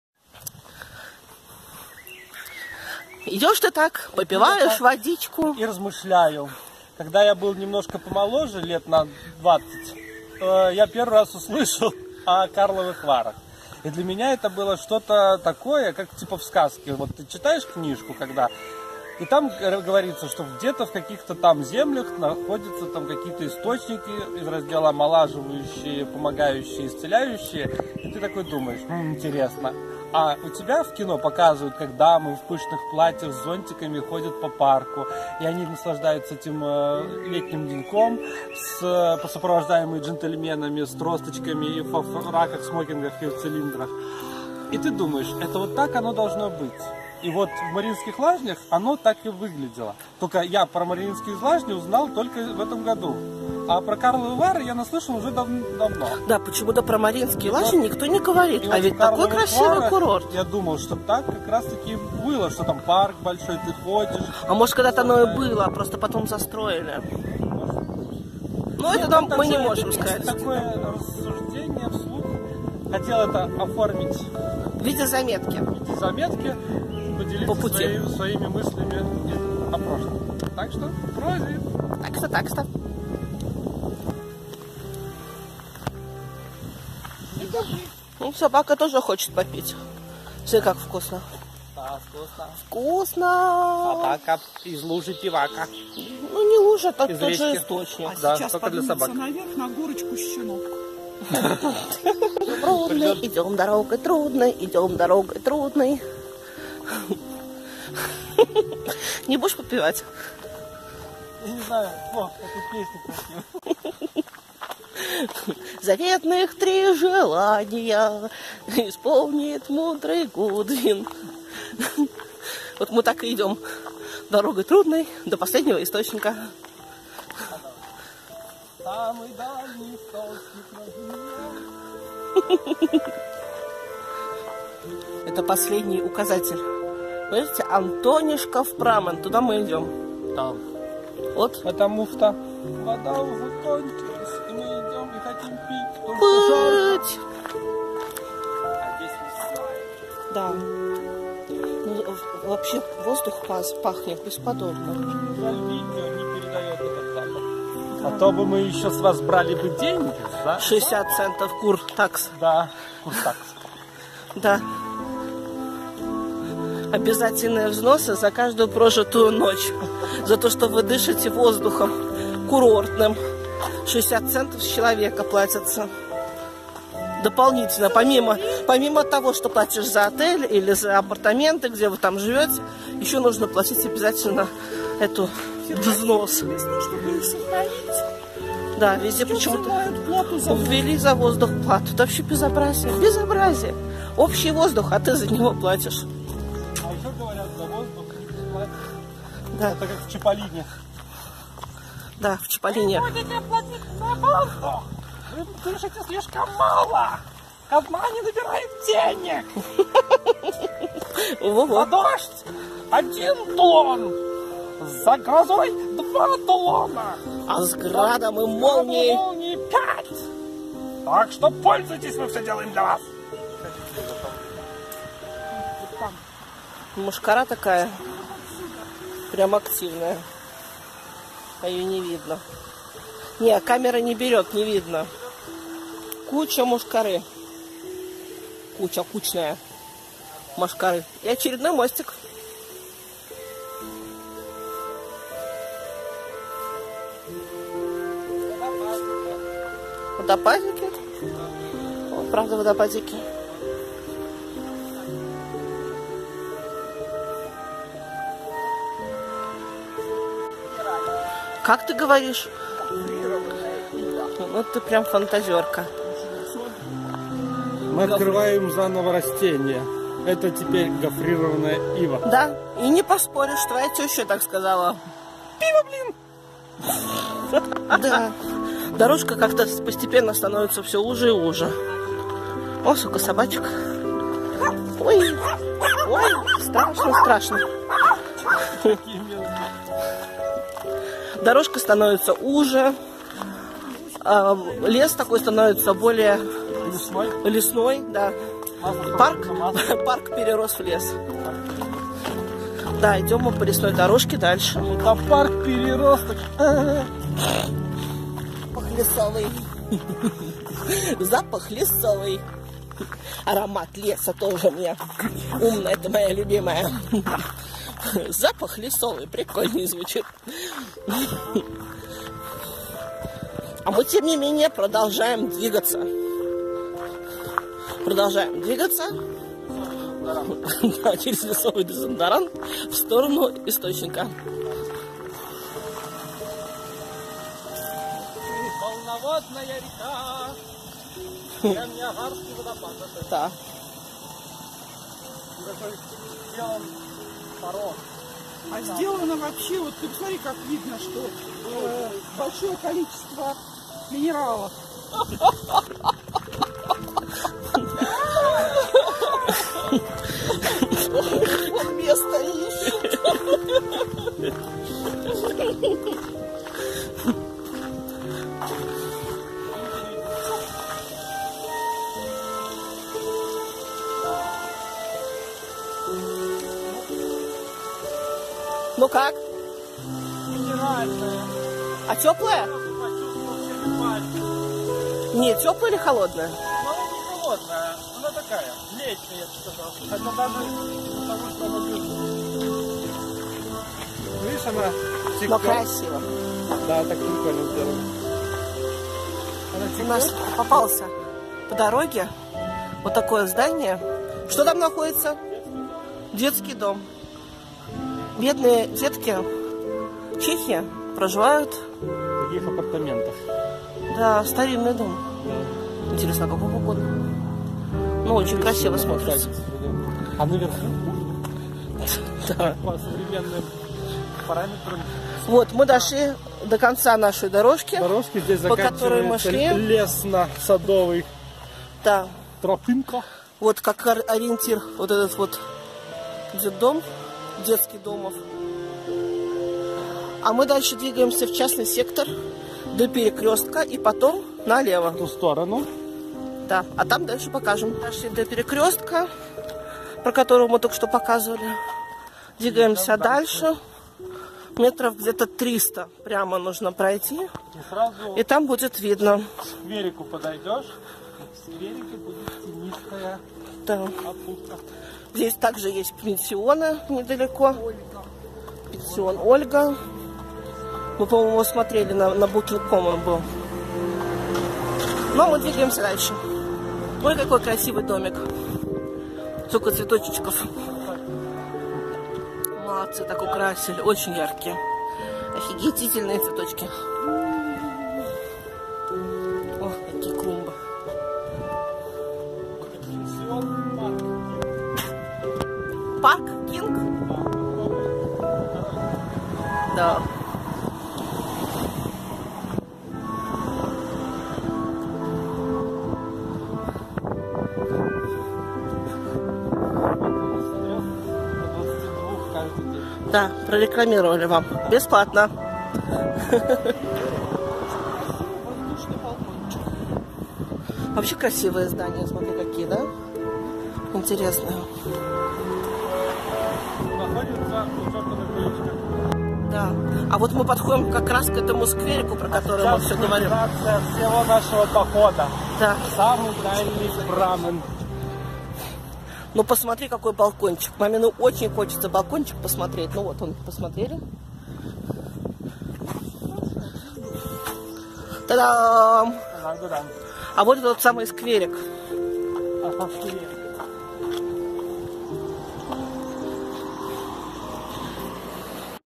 Путь к источнику Антонина. Марианские Лазни. Чехия.
Источник Антонина долгое время бил ключом безвестно. Своё название получил позже. В 1790 году в Ушовицих была построена церковь св. Антонина из Падовы и его название получил и источник, который во времена Первой республики был сюда проведён из выше расположенного леса.